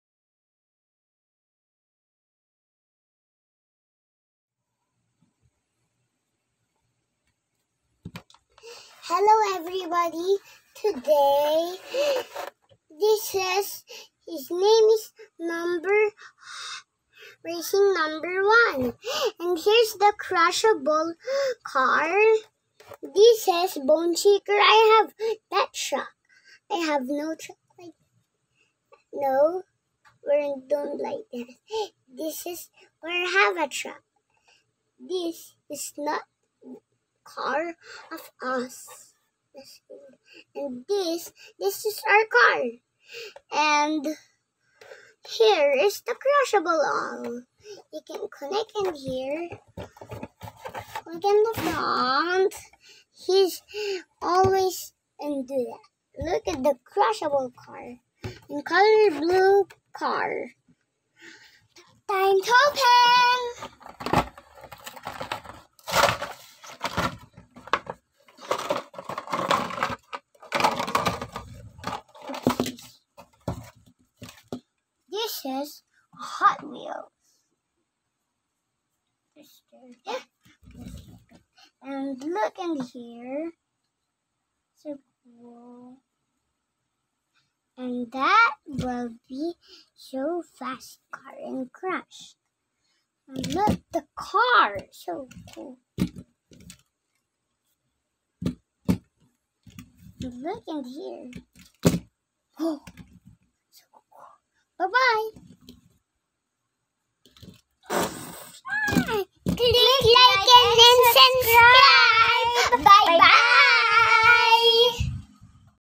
Hot Wheels Monster Trucks - Hot Wheels Racing 1 (Blue) - Unboxing
From the Hot Wheels Monster Trucks live... Hot Wheels Racing 1. It's chassis is actually Bigfoot 18 or Bigfoot 15, depending on the driver.